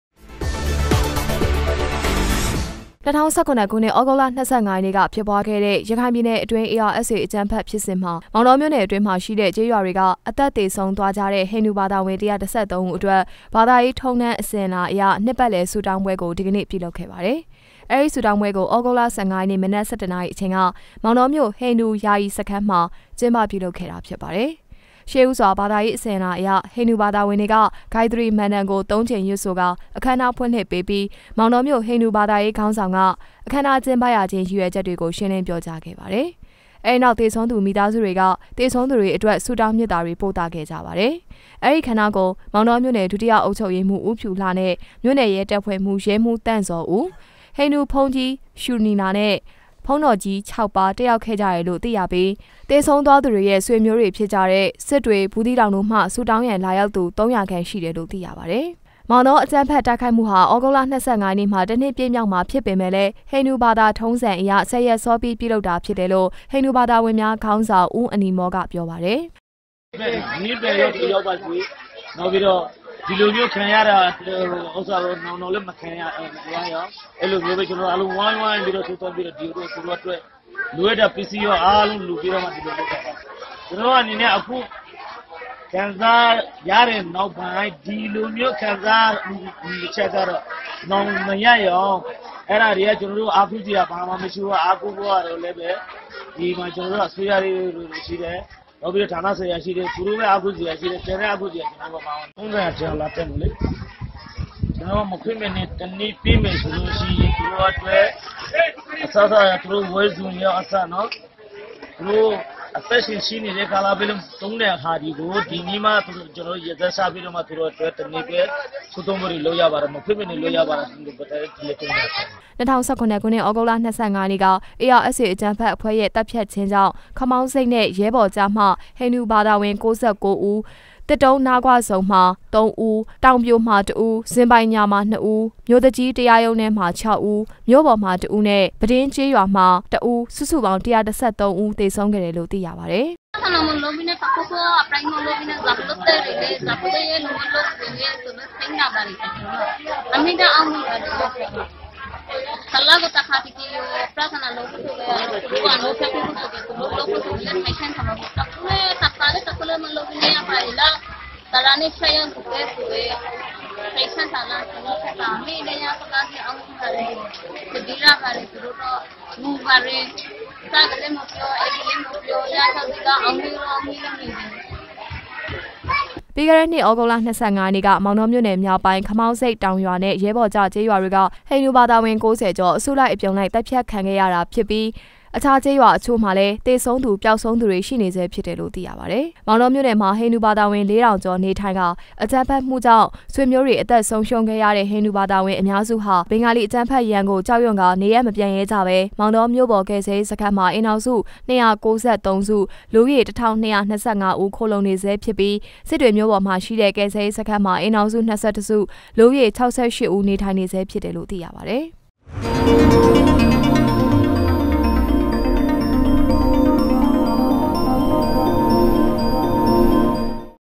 ARSAအၾကမ္းဖက္ျဖစ္စဥ္တြင္ ေမာင္ေတာျမိဳ႕ နယ္ အတြင္း ေသဆံုးခဲ့ရသည့္
၂၀၁၇ခုႏွစ္ျသဂုတ္လ၂၅ရက္ရခိုင္ျပည္နယ္အတြင္း ARSAအၾကမ္းဖက္ျဖစ္စဥ္တြင္ ေမာင္ေတာၿမိဳ႕နယ္အတြင္းရွိေက်းရြာမ်ားက  အသက္ေသဆုံးသြားၾကတဲ့ ဟိနၵဴဘာသာဝင္၁၁၃ဦးအတြက္ ဘာသာေရးထုံးတမ္းအစဥ္အလာအရႏွစ္ပတ္လည္ဆုေတာင္းပြဲကို ဒီကေန႕ျပဳလုပ္ခဲ့ပါတယ္။